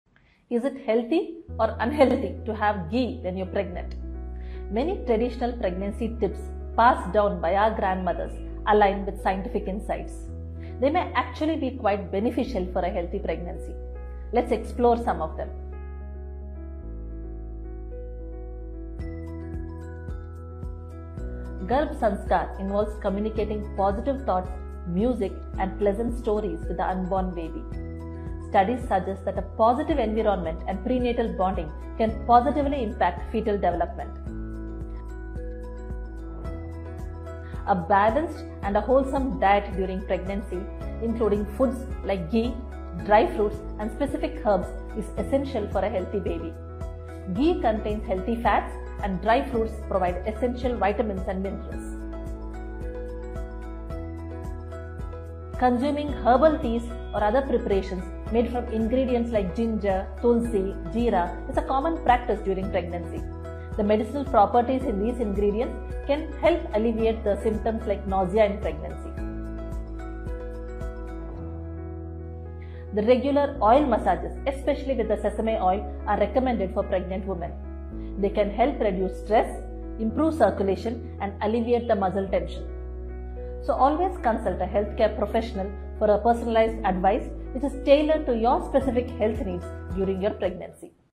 Grandma's tips for pregnancy | Best IVF Hospital in Hyderabad
We believe that healing begins within our homes, rooted in the practices passed down through generations.

While advancements in science and technology have undoubtedly expanded our fertility treatment options, we mustn't overlook the wisdom imparted by our elders. Incorporating both modern medical interventions and traditional practices can provide a comprehensive approach to fertility care. By honoring the timeless advice of our grandmothers alongside contemporary treatments, we can optimize our reproductive health journey.